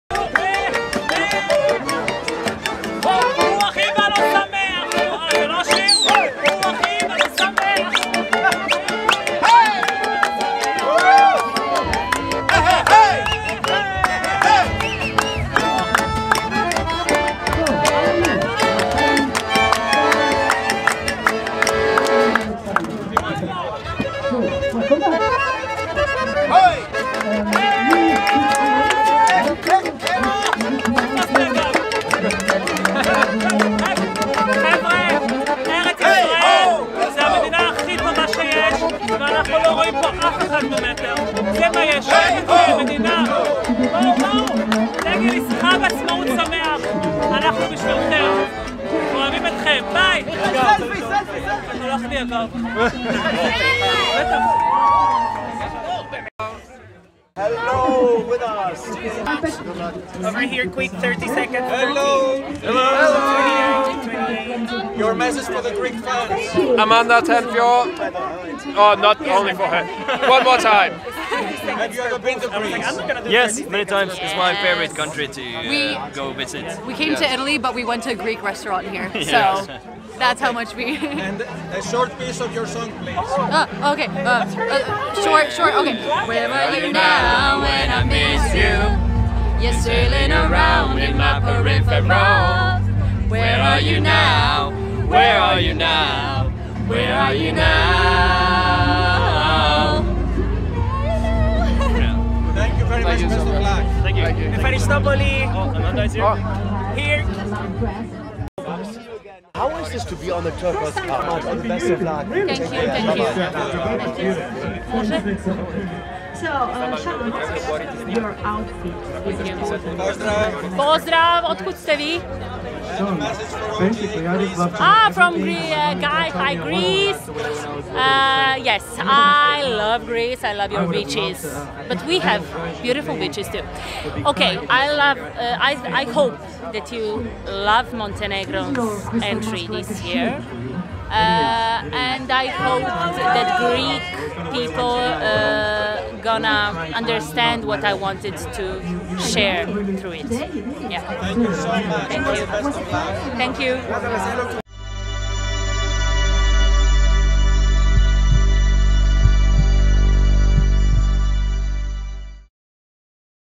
Eurovision 2022 : Opening ceremony - Turquoise carpet : OGAE Greece
O OGAE Greece στην τελετή έναρξης της Eurovision 2022 στο Τορίνο της Ιταλίας